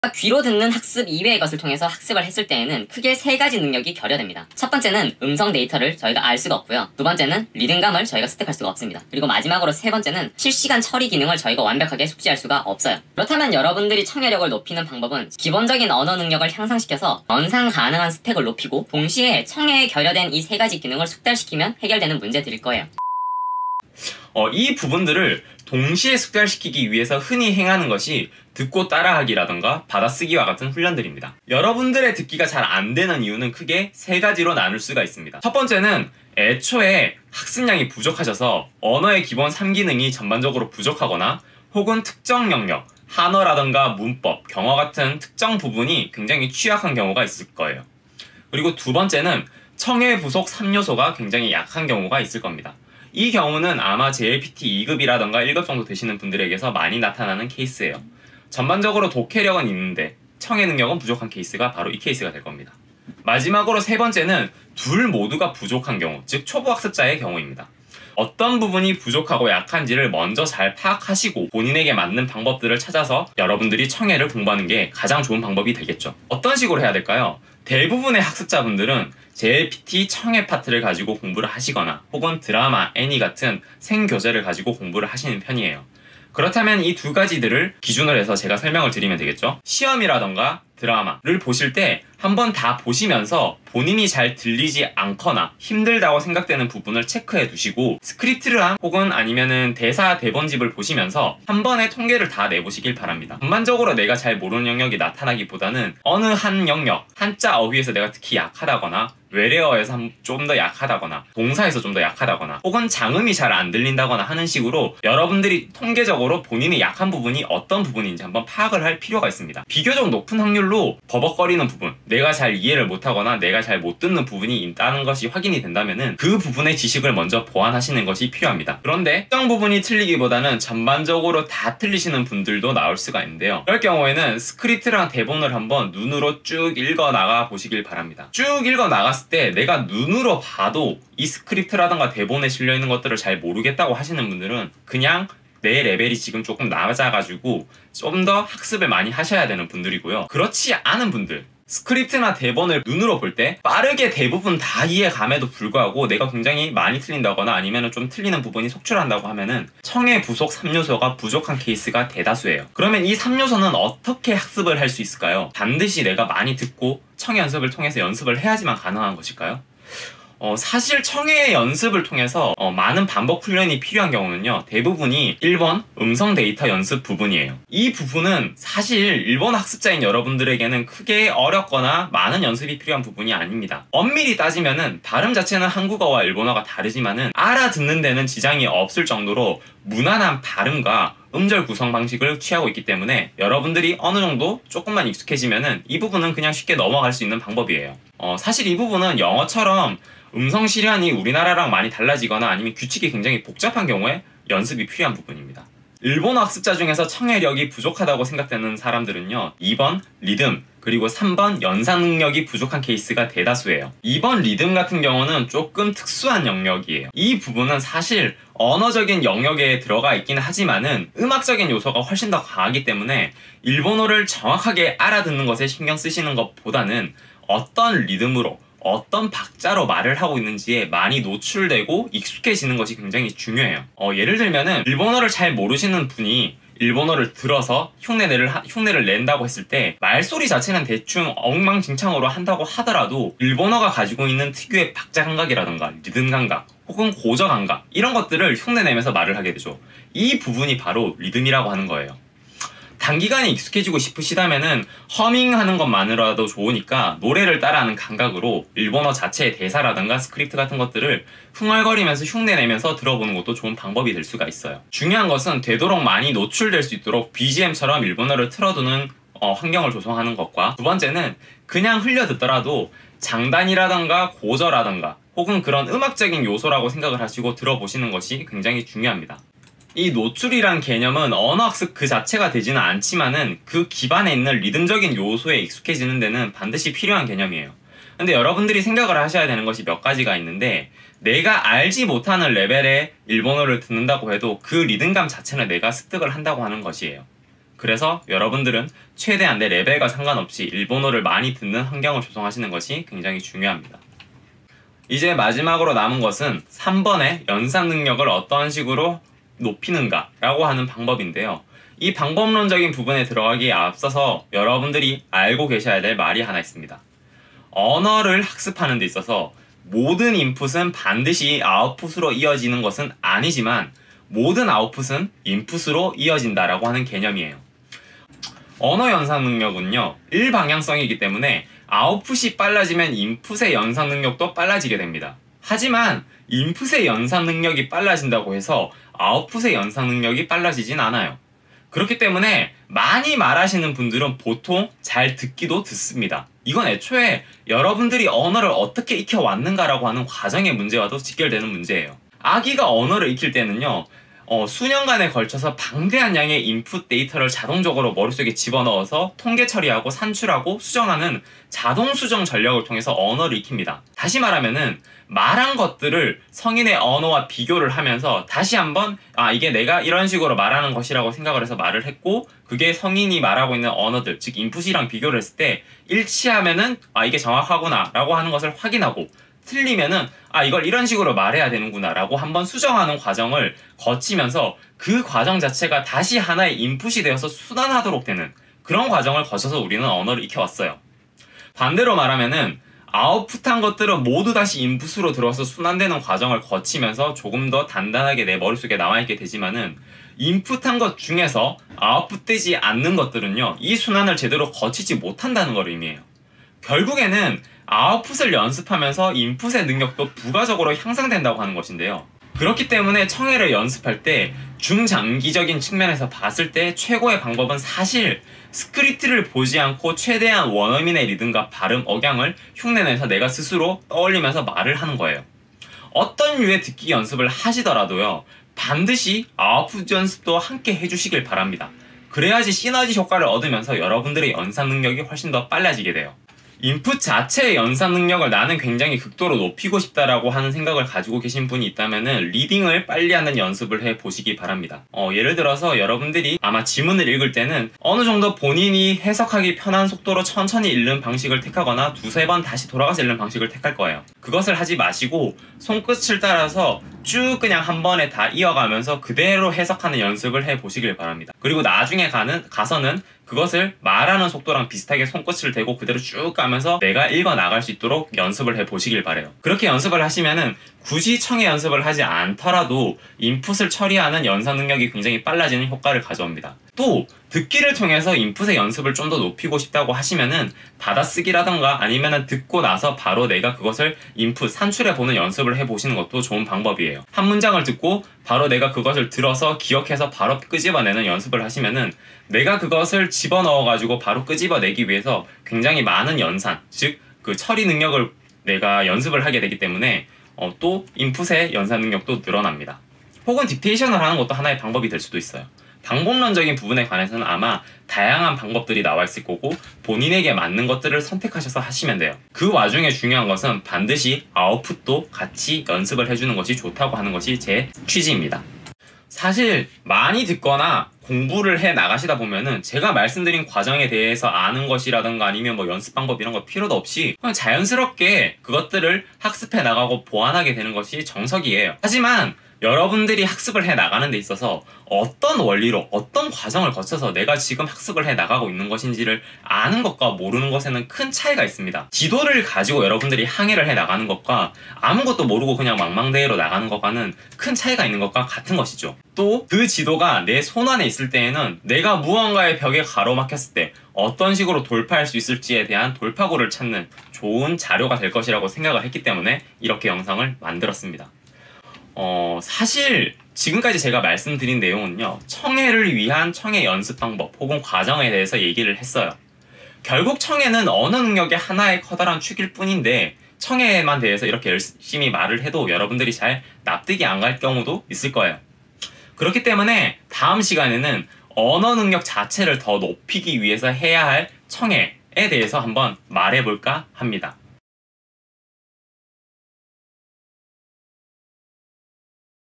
[Ryu-の辛口] #10 청해의 비밀_2편. 청해 공부법 찾기 전에 반드시 보셔야 할 진실
그냥 이 공부법 저 공부법을 본다고 되는게 아닙니다. 그냥 막 듣는다고 갑자기 실력이 오르지 않습니다. 헤매시는 분들을 위한 영상입니다.